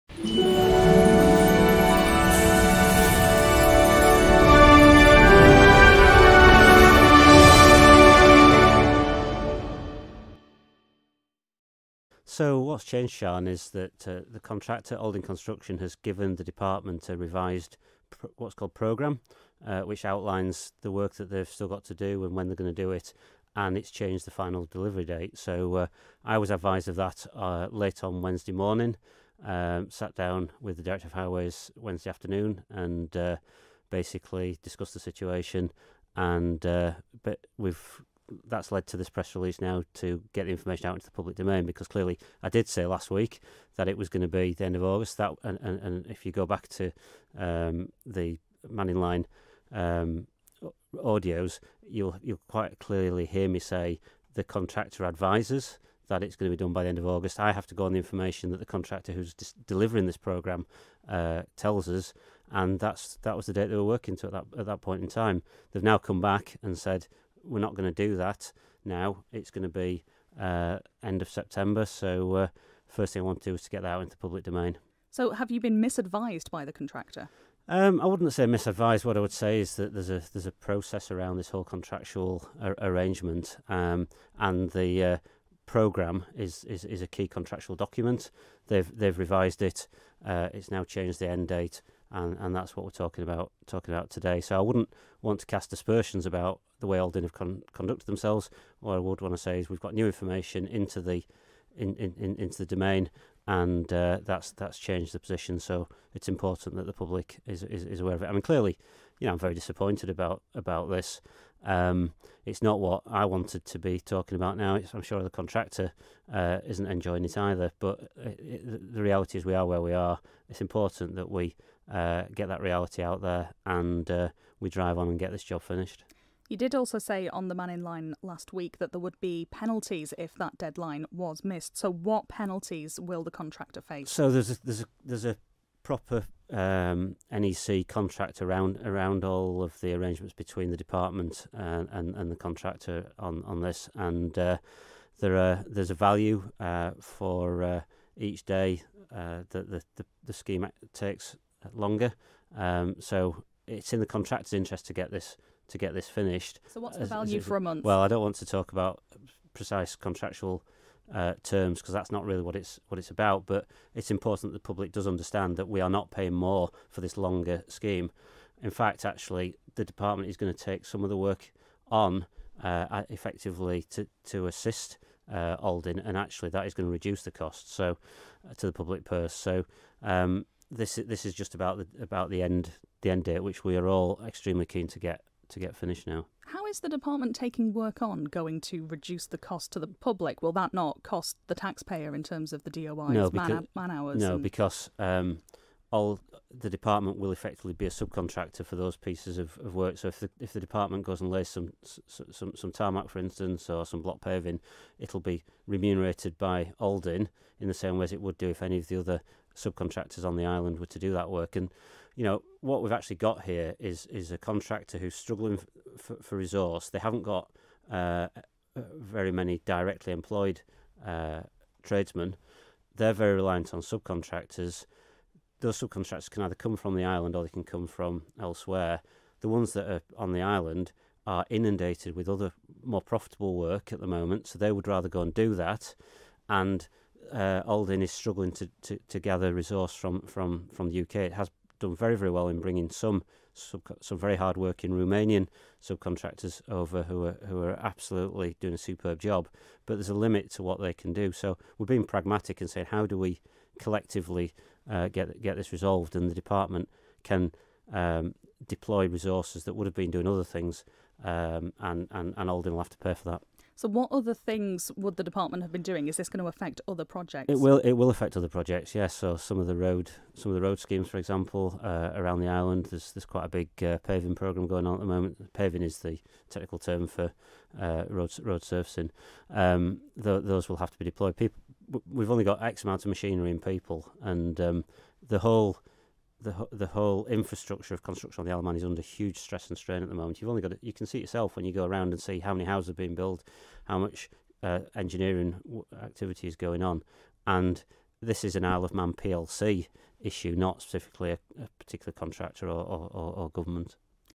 Delay to completion of Douglas prom
Infrastructure Minister Tim Baker joined Ben and Sian on Manx Radio Breakfast following this morning's announcement that the roadworks on Douglas promenade won't be finished next month